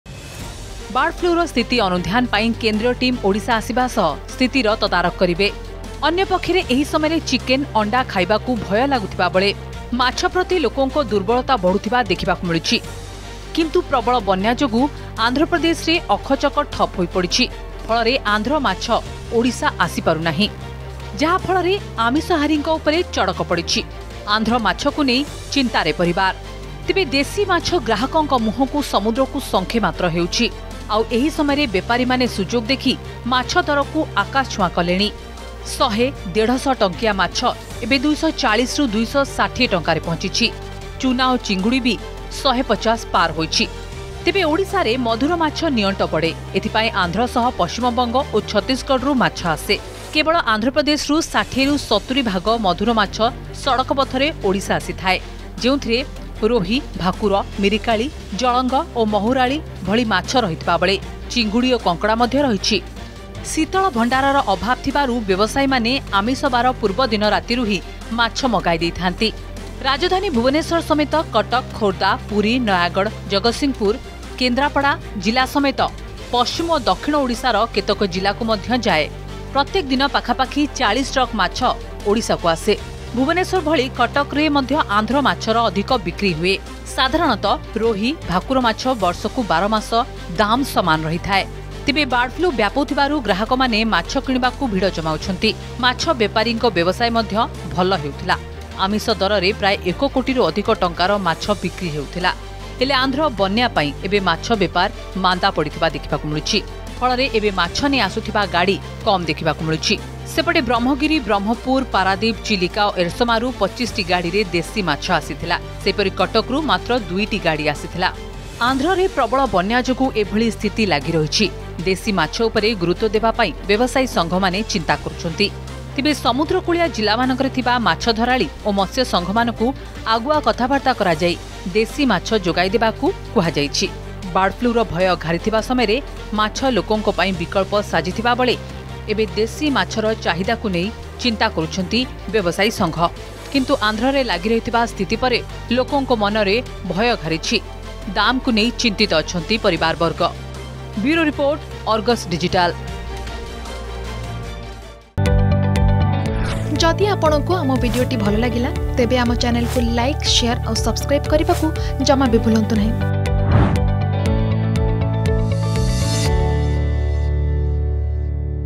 ଆନ୍ଧ୍ରରେ ବନ୍ୟା, ଆସିପାରିବନି ମଧୁର ମାଛ ! Andhra Pradesh Fish Demand || Flood Impact
ବାର୍ଡ ଫ୍ଲୁ ବେଳେ ନୂଆ ଟେନସନ୍ । ଆନ୍ଧ୍ରରେ ବନ୍ୟା ପାଇଁ ମାଛ ବେପାର ମଧ୍ୟ ମାନ୍ଦା ହେବାକୁ ଯାଉଛି